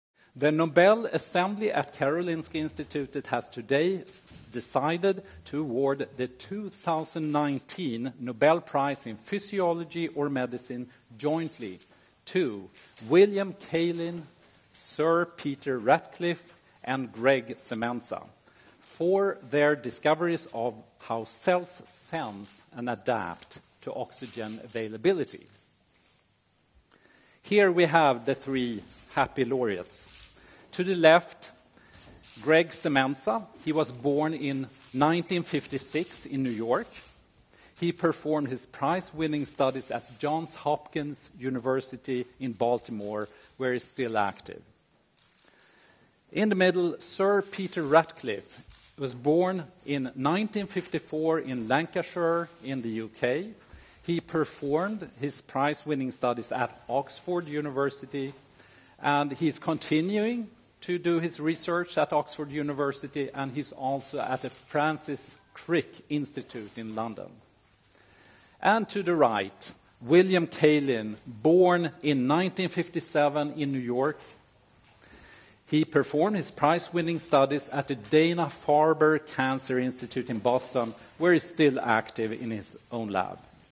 Nobel Prize In Medicine Awarded To 3 Scientists For Oxygen Research | TIME
The 2019 Nobel Prize for Physiology or Medicine has been awarded to scientists William G. Kaelin, Jr, Peter J. Ratcliffe and Gregg L. Semenza.